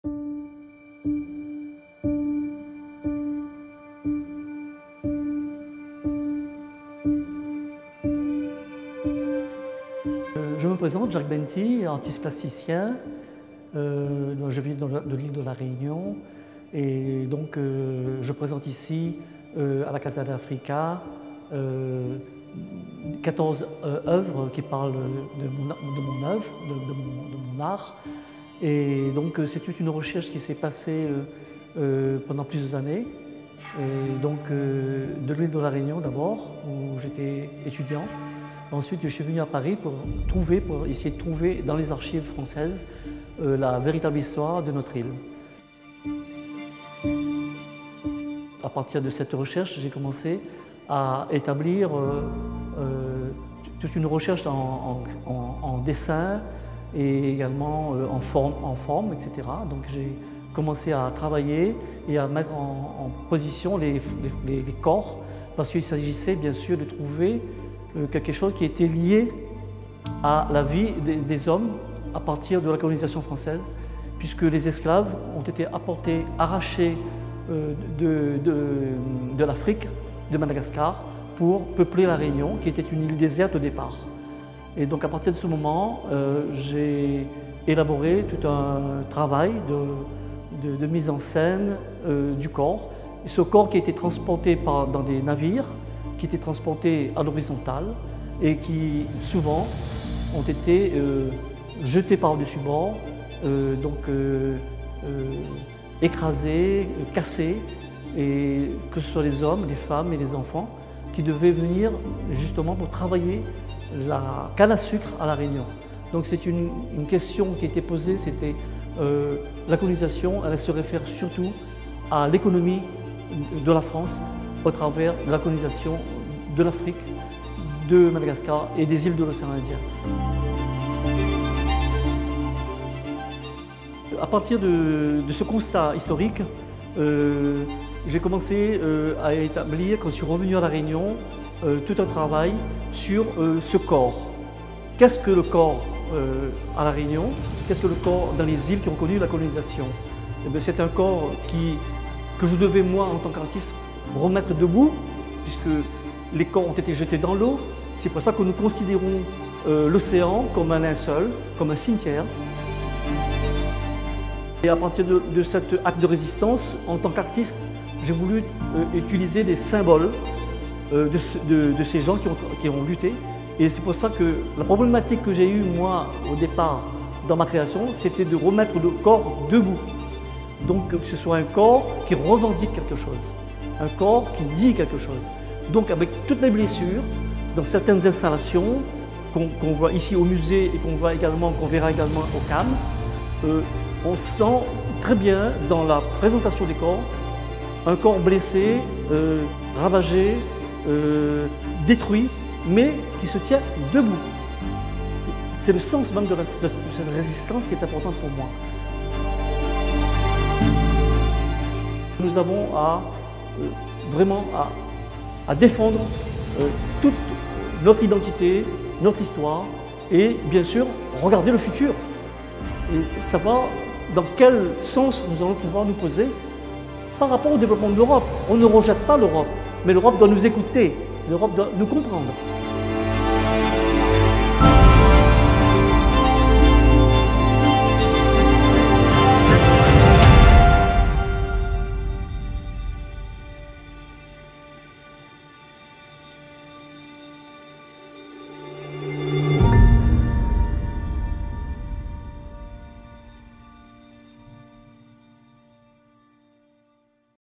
EXPOSICIÓN DE JACK BENG-THI EN CASA ÁFRICA
Exposición del artista Jack Beng-Thi que se celebra del 19 de octubre de 2018 al 24 de febrero de 2019 en Casa África y del 17 de octubre de 2019 al 26 de enero de 2020 en el CAAM.
Casa África colabora con el Centro Atlántico de Arte Moderno (CAAM) para acoger de manera compartida la obra de Jack Beng-Thi, reconocido artista multidisciplinar de formación escultórica. Su extensa obra, de amplio calado histórico, social, político y ecológico, se inscribe en un profundo compromiso con las culturas de las diversas geografías en las que ha desarrollado su trabajo artístico. Nacido en la Isla de la Reunión, refleja en su obra el crisol de culturas que esta isla recoge en el Océano Índico. Su familia es de origen indio, chino-vietnamita, africano y europeo y desde estas referencias culturales ha emprendido un viaje permanente como artista-antropólogo contemporáneo. Ver más